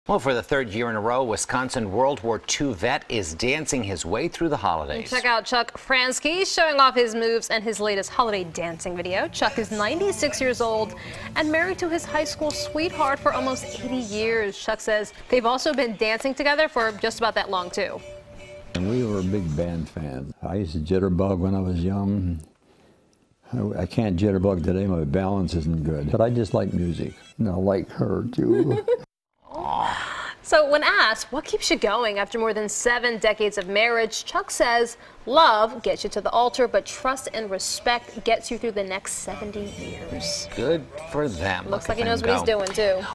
96-Year-Old WWII Veteran Dances In Holiday Video
Chuck Franzke has been dancing with his high school sweetheart for nearly 80 years.